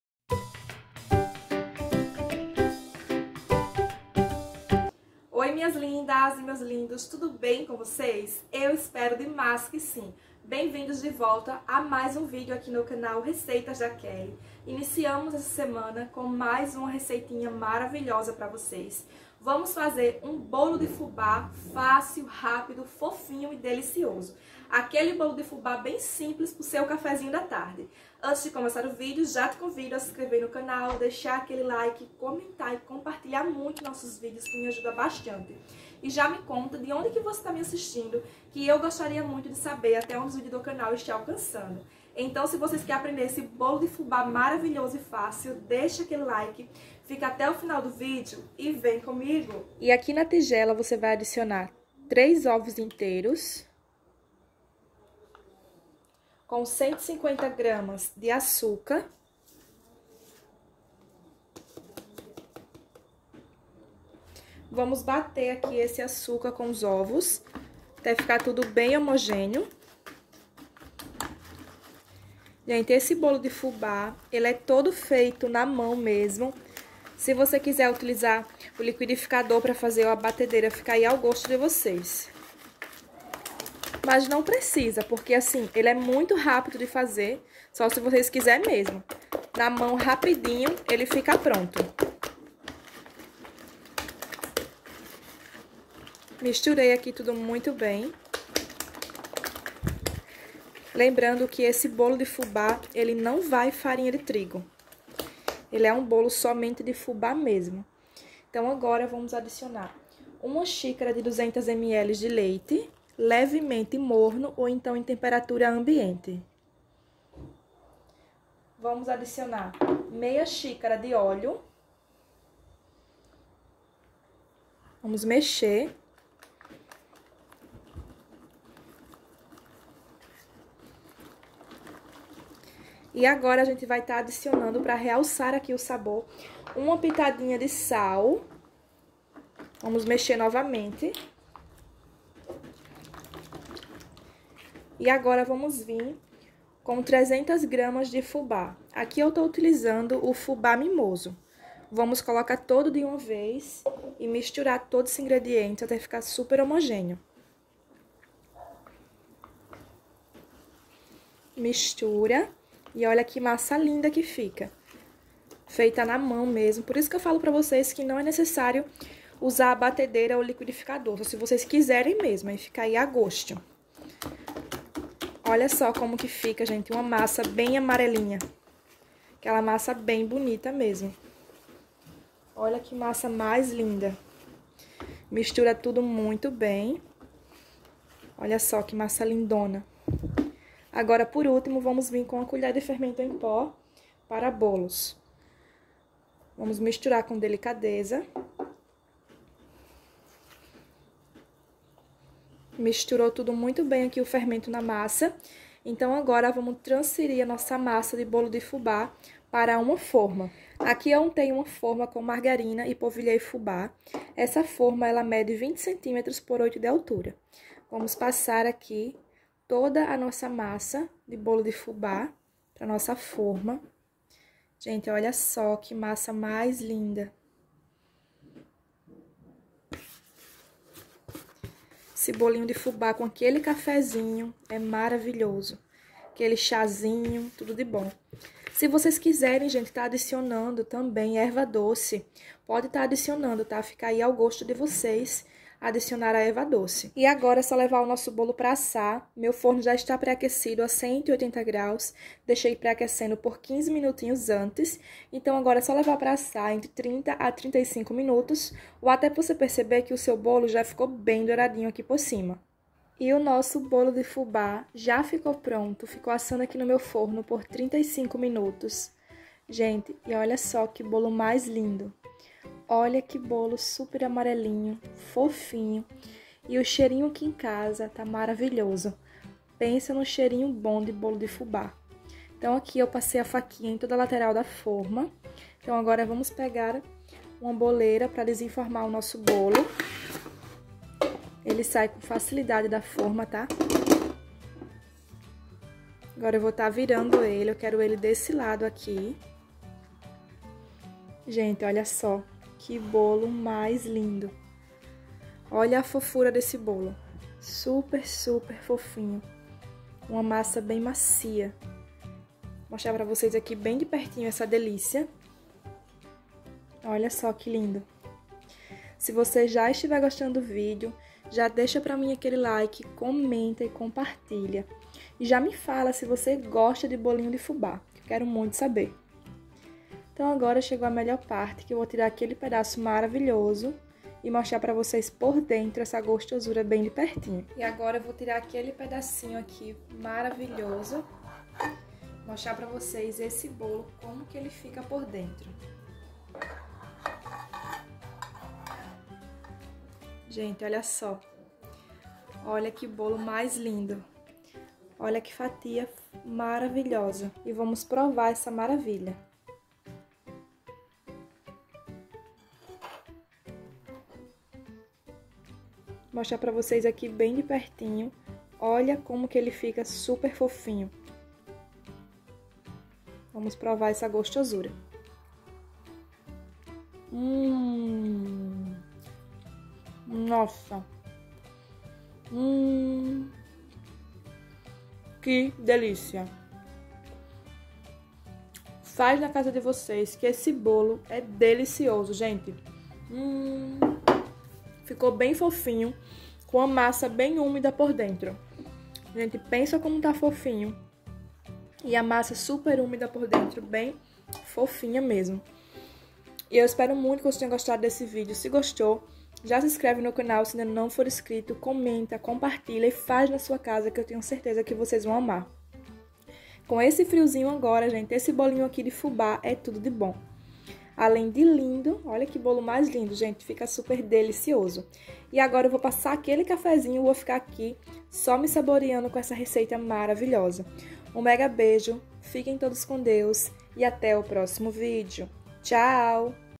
BOLO DE FUBÁ SEM TRIGO FÁCIL PRÁTICO E RÁPIDO FOFINHO PARA SEU CAFÉ DA MANHÃ
Oii minhas lindas e meus lindos, tudo bem com vocês eu espero muito que sim e hoje vamos fazer um Delicioso bolo de fubá maravilhoso, espero que vocês gostem 💗

ingredientes são

300 gramas de fubá
150 gramas de açúcar
3 ovos
1/2 de óleo
200 ml de leite
uma pitada de sal
1 colher de fermento de bolo

Modo de preparo junto com todo passo a passo assita ao vídeo acima com a Kelly 💖

Deus abençoe todos vocês 🙏 bjs